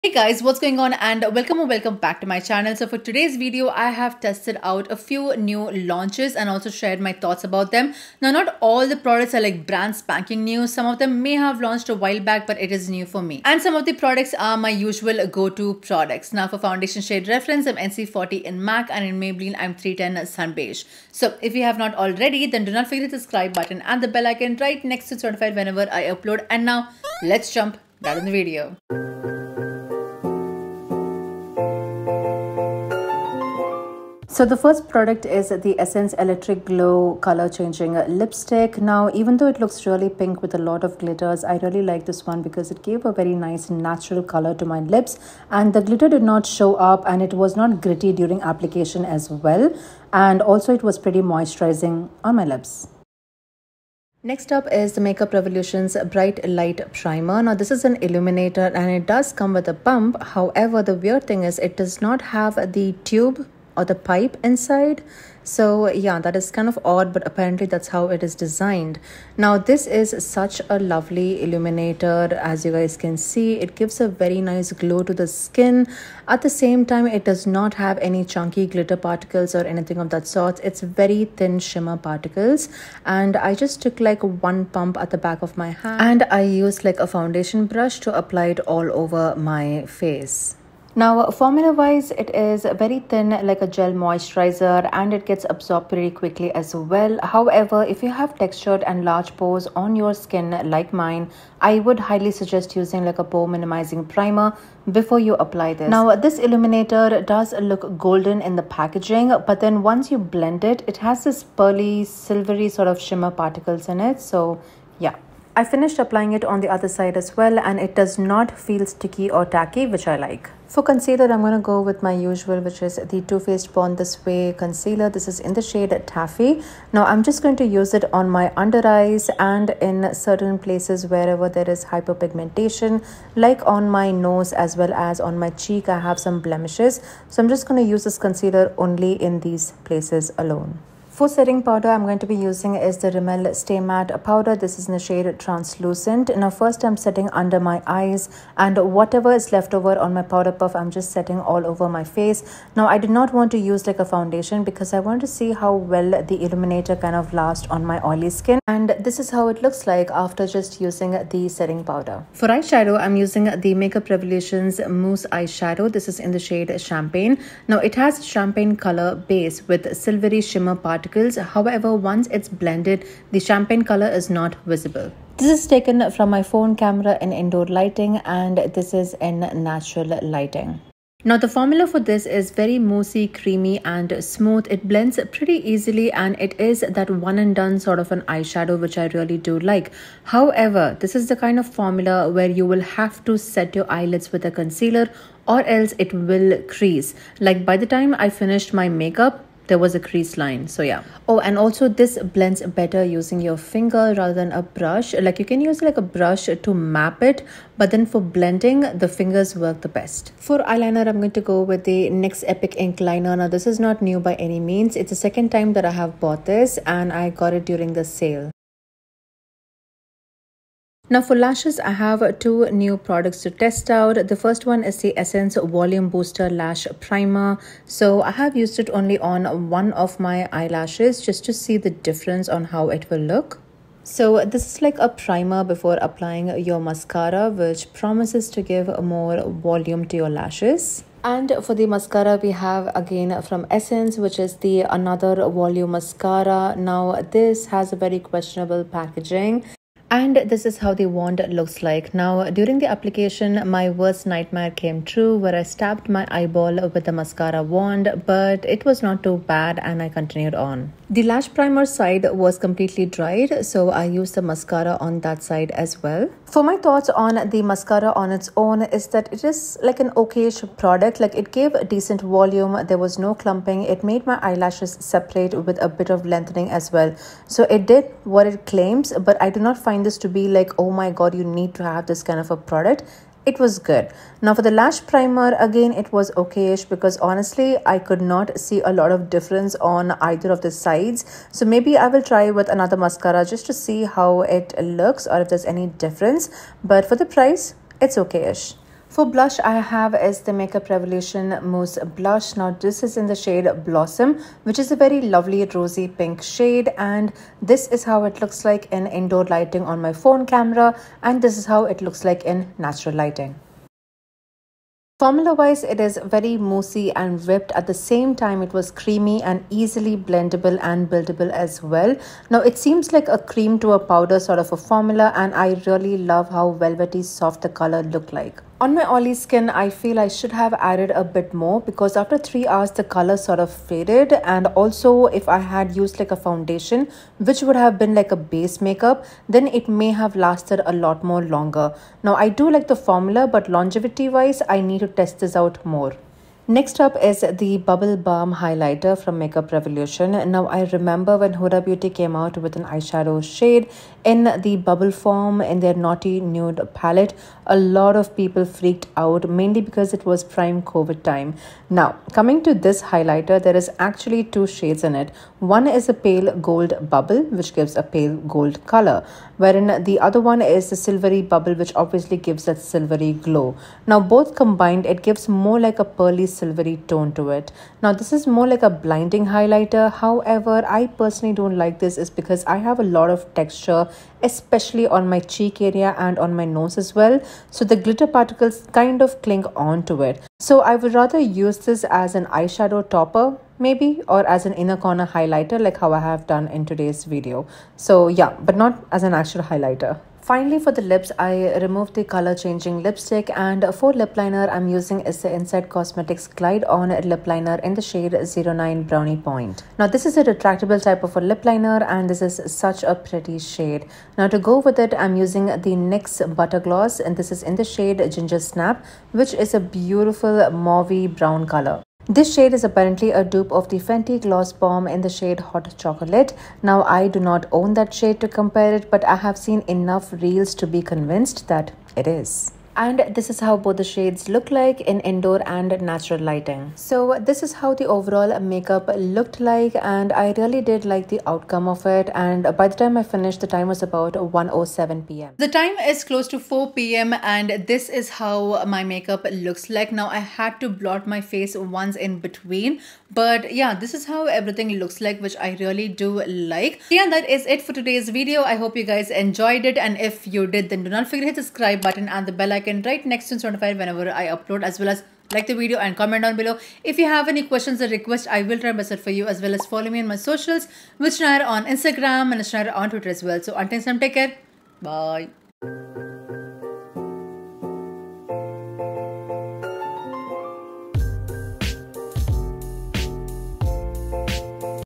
Testing NEW LAUNCHES (makeup revolution, insight cosmetics, essence & NYX)
Hey Guys,

Hello!! Here is a video where I test out a bunch of NEW Makeup.

--- Products Used —

NYX butter gloss - ginger snap (from Amazon)